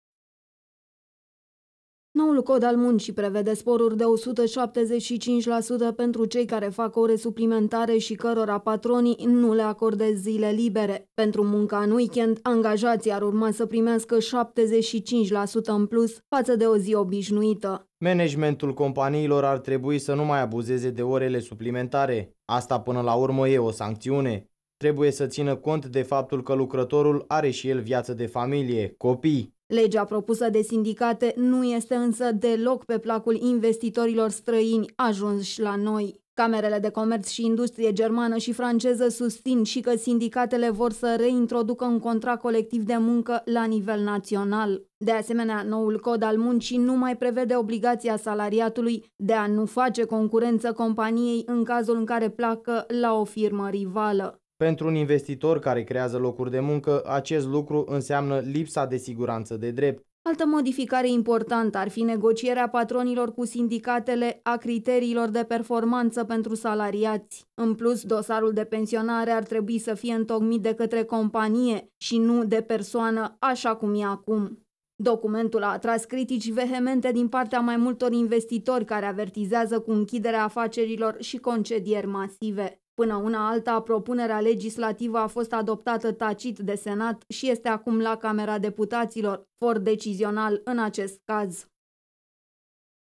Noul cod al muncii prevede sporuri duble pentru zilele şi orele suplimentare
Salariaţii care muncesc peste orele de program sau în weekend vor fi recompensaţi pe măsură. Până ca angajaţii să primească bani în plus, mai este, însă, un pas. Modificările propuse de sindicate la Codul Muncii au trecut de Senat, iar acum sunt dezbătute la Camera Deputaţilor, care este for decizional.